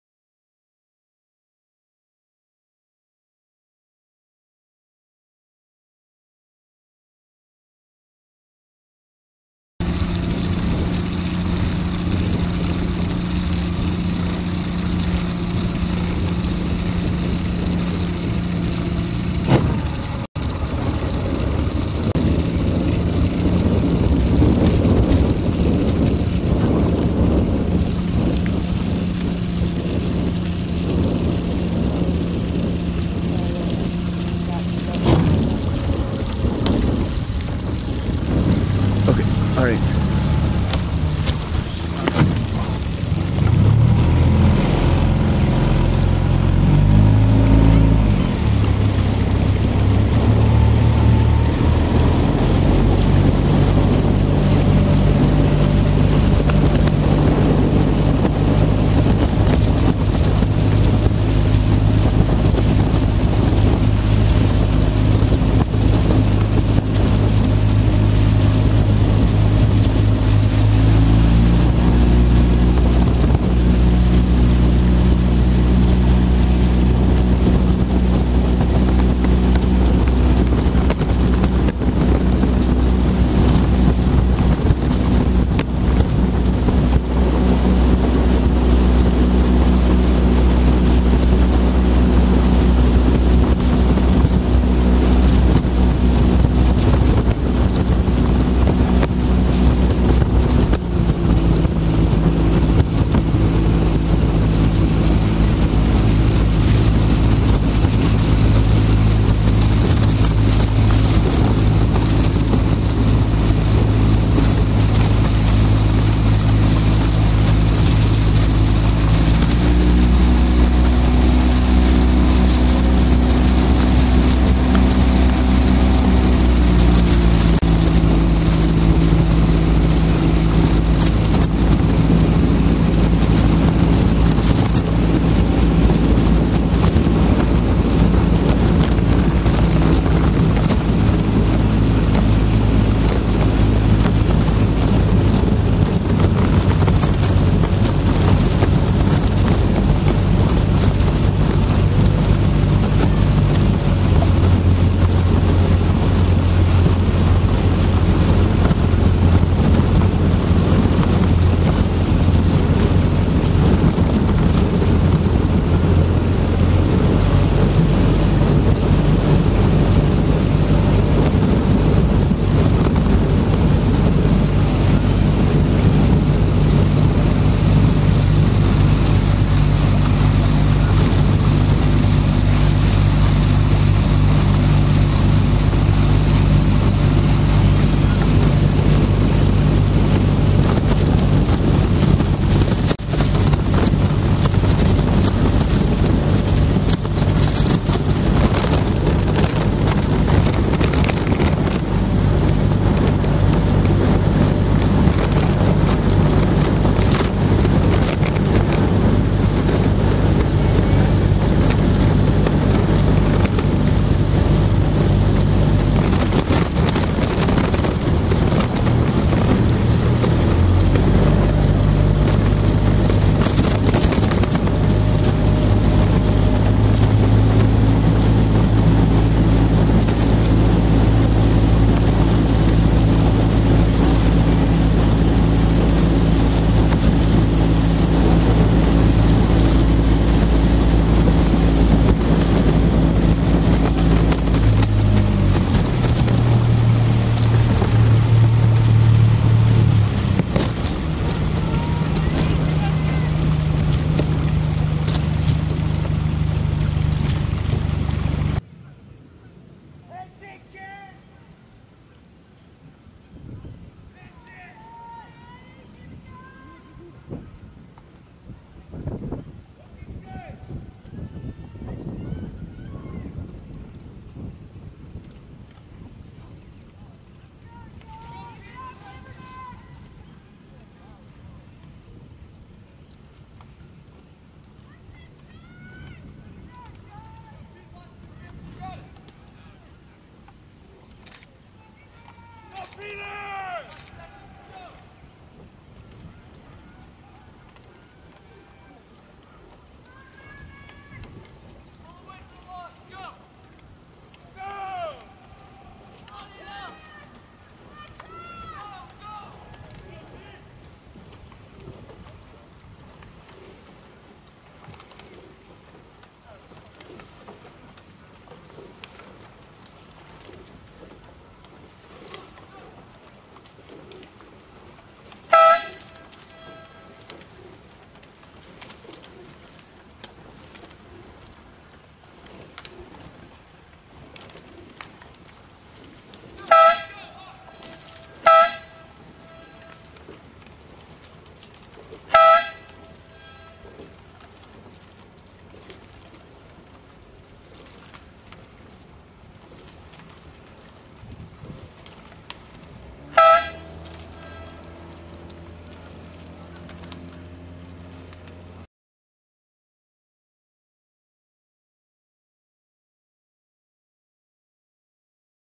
2008 US Rowing NSR #1 Men's Single Sculls Final
USRowing is the national governing body for the sport of rowing. To learn more about USRowing or to view more video clips, please

1. Unaffiliated - Ken Jurkowski, 08:02.013

2. Penn A.C. Rowing Association - Andrew Liverman, 08:13.169

3. Bantam Boat Club - Peter Graves, 08:14.940

4. Vesper Boat Club - Joshua Brown, 08:18.866

5. New York Athletic Club - James Dietz, 08:29.965

6. Atlanta Rowing Club - Jordan Smith, 08:52.961